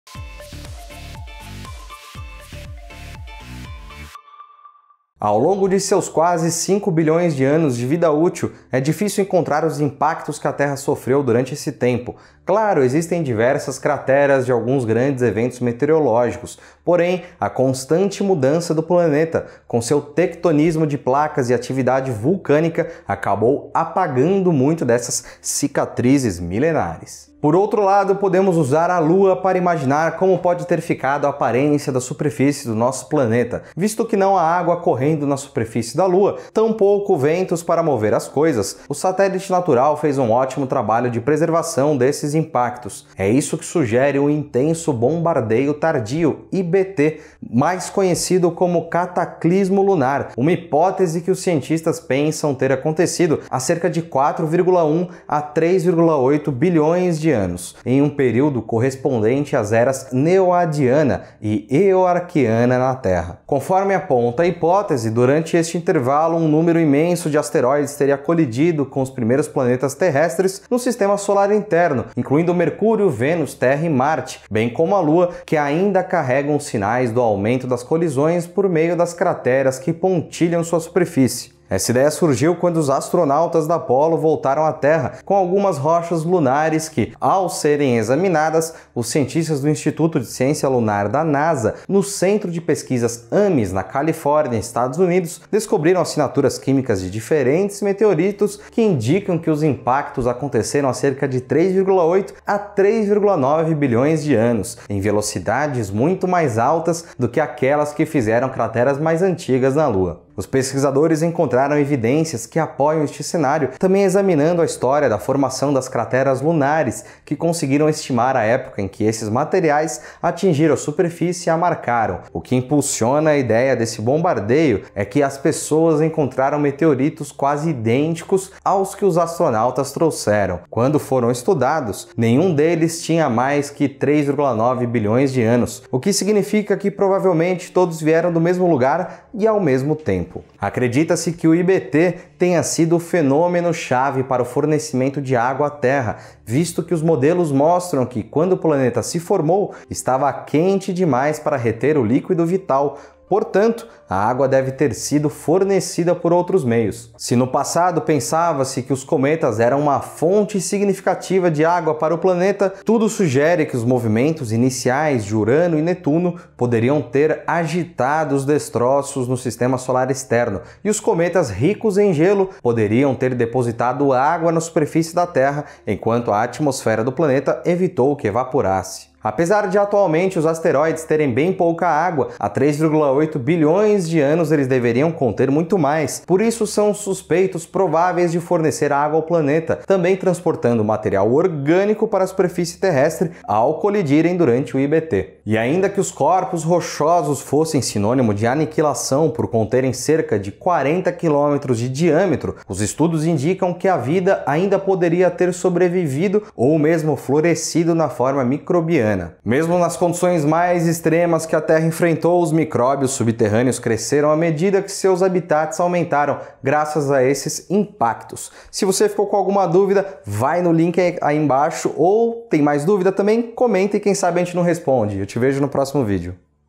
Intenso Bombardeio Tardio: o fenômeno que aniquilou a Terra - TecMundo Drops
Ao longo de seus 4,54 bilhões de anos de vida útil, é difícil encontrar os impactos que a Terra sofreu durante esse tempo. Claro, existem diversas crateras de alguns grandes eventos meteorológicos, porém a constante mudança do planeta, com seu tectonismo de placas e atividade vulcânica, acabou apagando muito dessas cicatrizes milenares. Confira!

CRÉDITOS
Apresentação e roteiro: Felipe Payão
Imagens (de casa): Felipe Payão
Edição de vídeo: Rafael Nogoceke
Motion Graphics: Gustavo Silva
Arte da thumb: Fernando Perazzoli